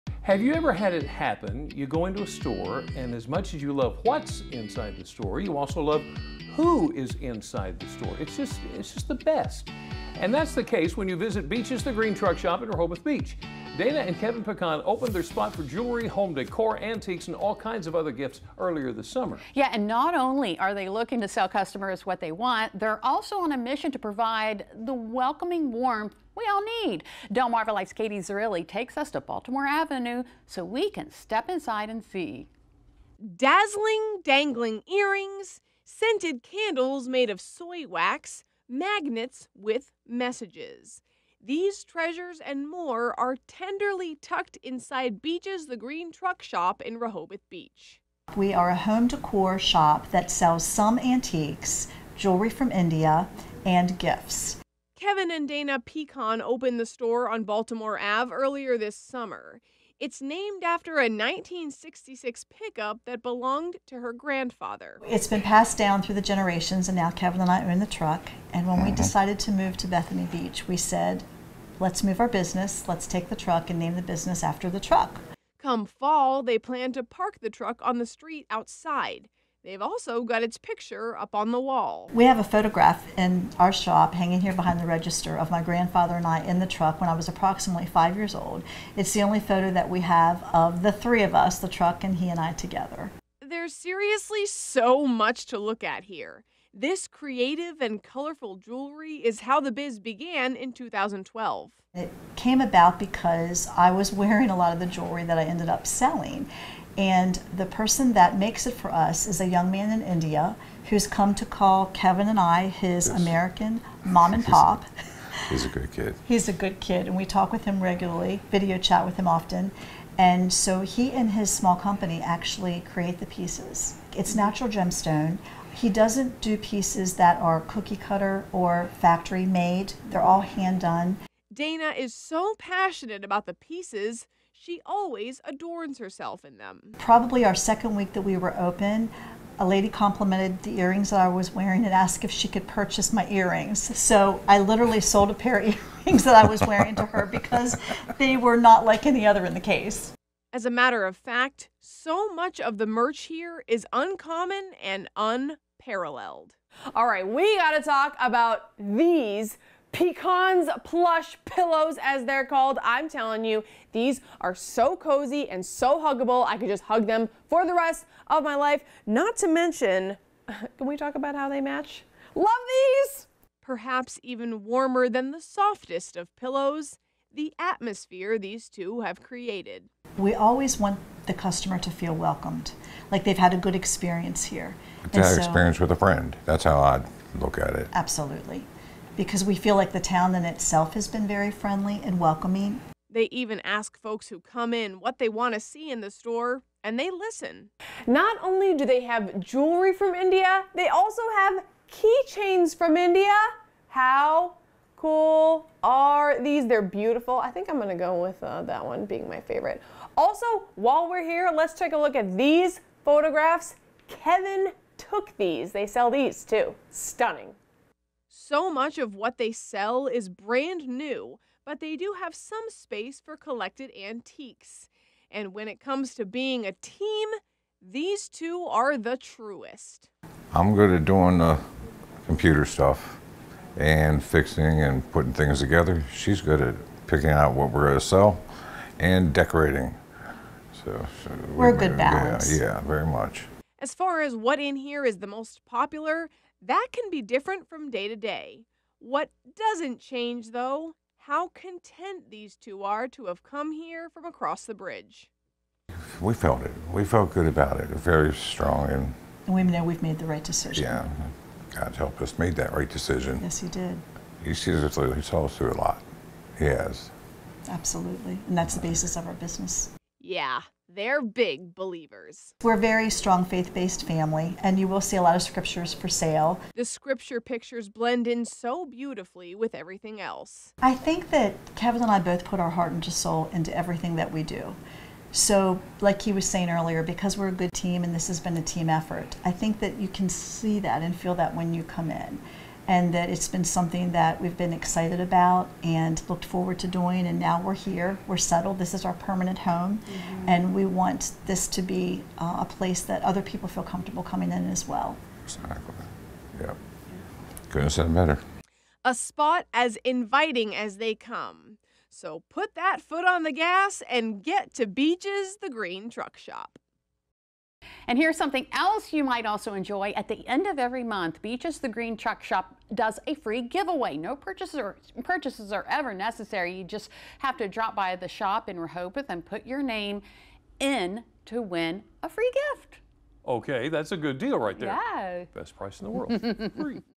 Katie Takes A Trip To Rehoboth Beach To Visit Beaches The Green Truck Shop
Katie brings us to a cool shop located in Rehoboth Beach that is bursting with the most unique gift ideas you can find on the peninsula.